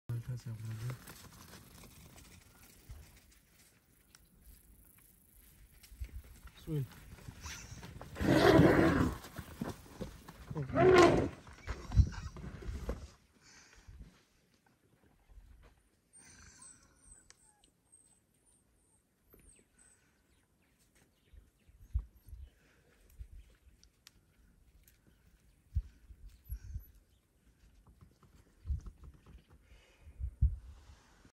Elephant vs Wild dogs - African Bush Camps
Amazing sighting of an elephant chasing a pack of Wild dogs. 🐘

Video taken at Lolebezi in the Lower Zambezi National Park, Zambia.